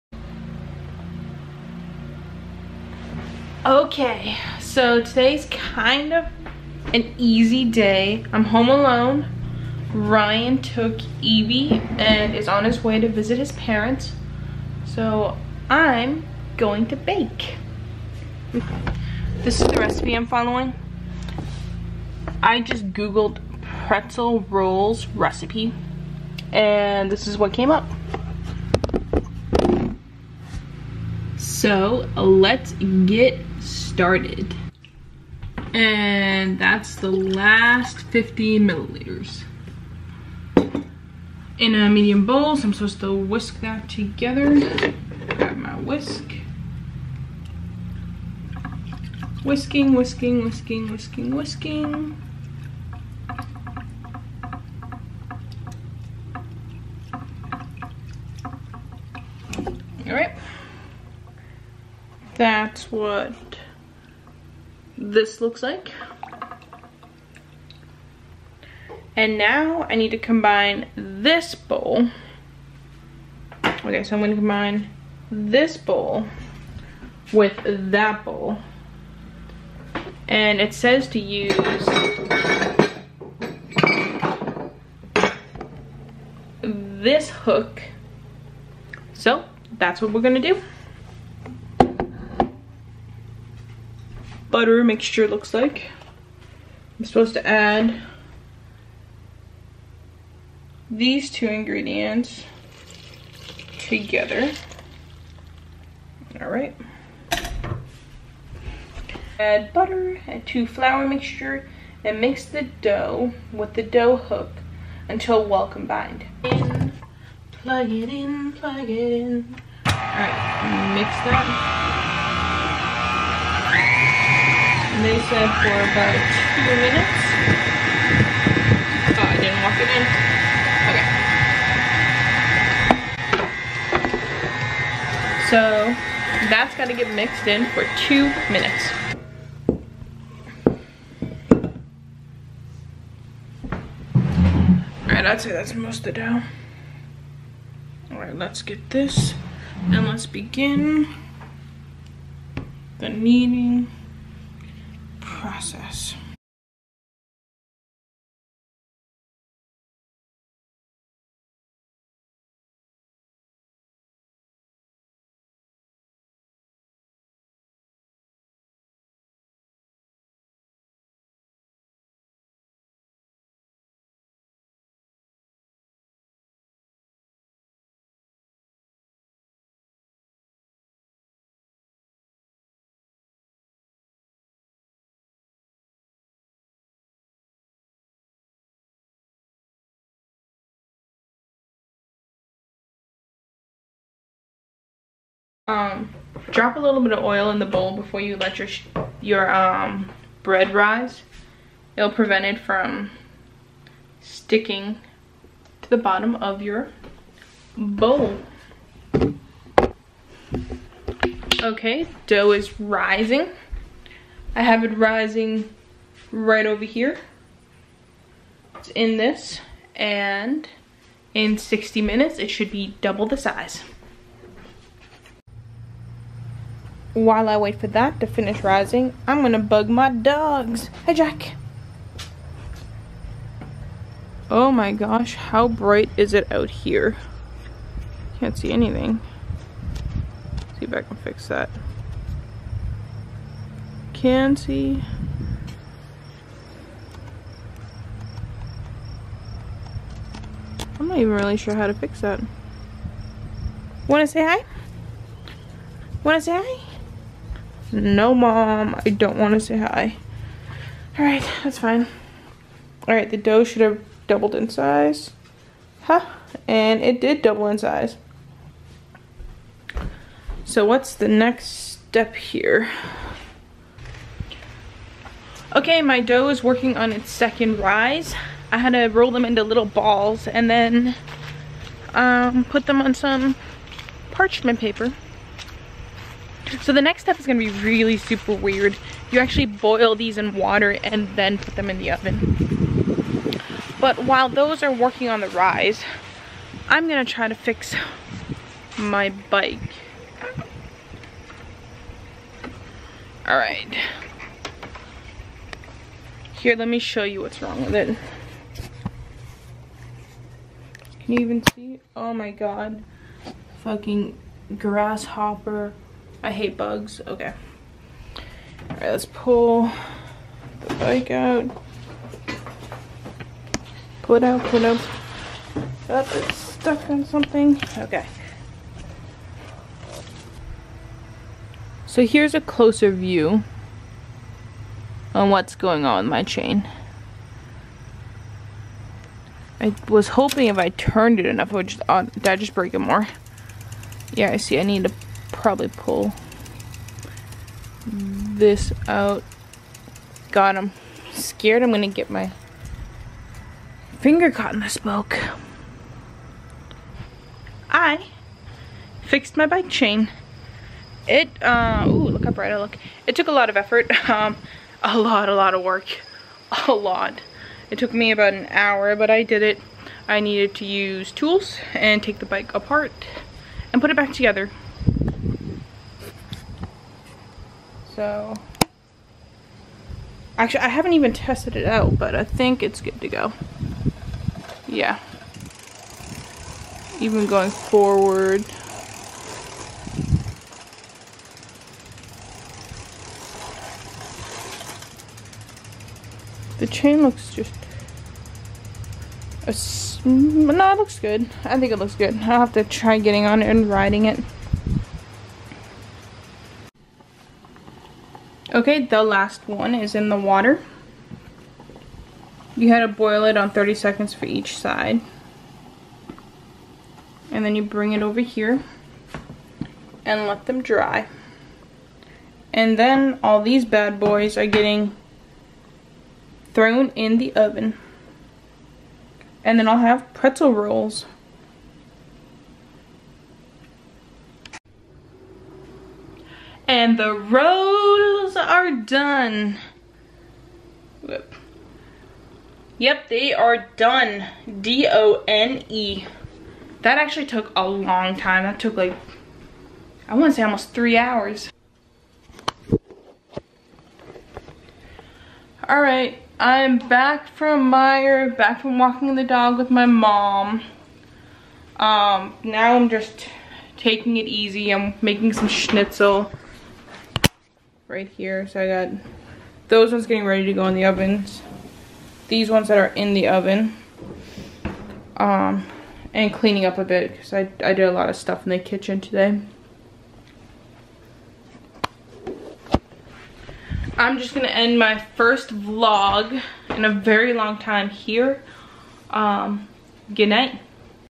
First Vlog | I made Pretzel Rolls!
This is my first blog ever! Today I made some pretzel rolls and fixed my bike. It was a pretty easy and relaxing day